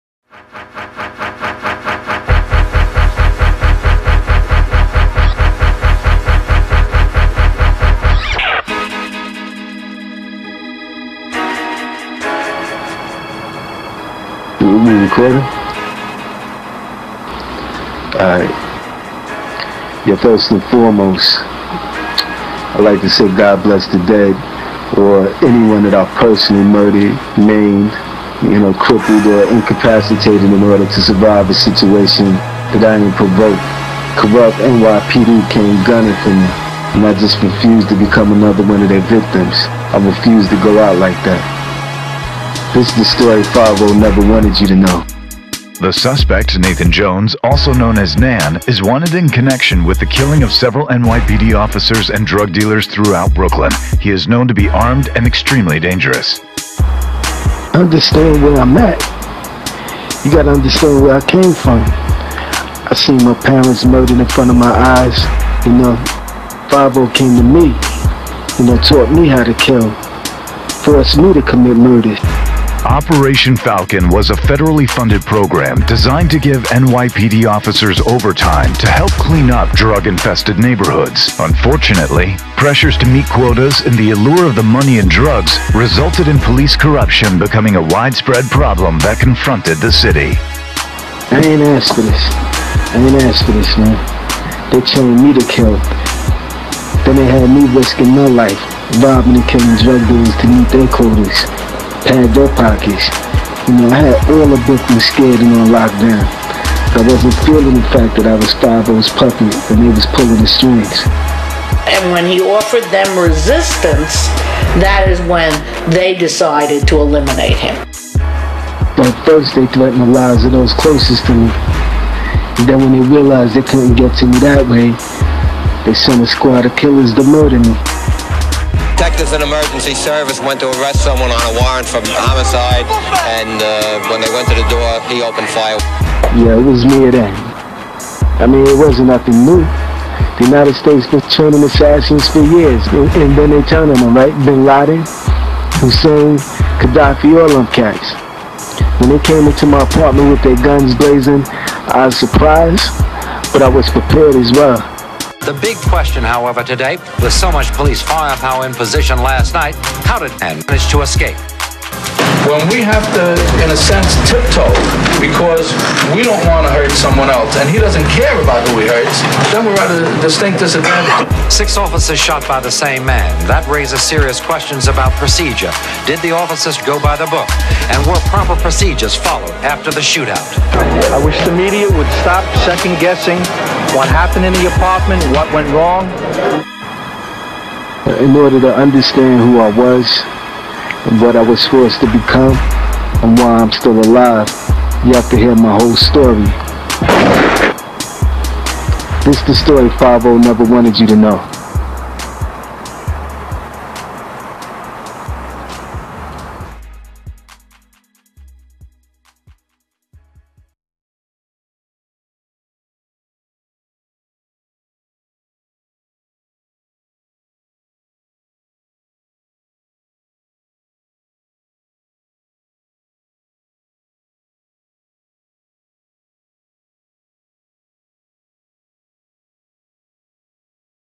Nan: The Trifling Times of Nathan Jones Book Video
Video trailer for one of the hottest books on the streets...NAN: The Trifling Times of Nathan Jones, is the riveting story of a troubled black male from Brooklyn, New York that was forced underground after warring with a corrupt NYPD unit in the 1980's.  Fortunately for Nathan Jones (also known as Nan), he survived the attempt to end his life, and lived to tell his tale.

But, to understand what Nan became, you have to take the time to fully understand what he was forced to become. At the tender age of thirteen, Nan witnessed the brutal murders of his parents, at the hands of a drug crazed PCP addict.  This tragic event resulted in his childhood being stolen from him, and what remained of his teenage years being spent in an orphanage.

As he struggled to deal with his loss, a police officer named Mark Carson entered his life and became the father figure that he desperately needed. He was also introduced to another youth, named Joseph Hayes who happened to be an orphan as well. Their bond was immediately cemented, when they both discover that they share an unfortunate similarity.  On the same fateful night, the same man murdered both of their parents.

The two youths became inseparable blood brothers, with Mark Carson serving as their mentor and surrogate father. Unfortunately, as the prospects for the future began to look more promising, tragedy rears its ugly head once again. This time, Nan is reeled into a deadly game of cat and mouse that threatens to destroy everything around him.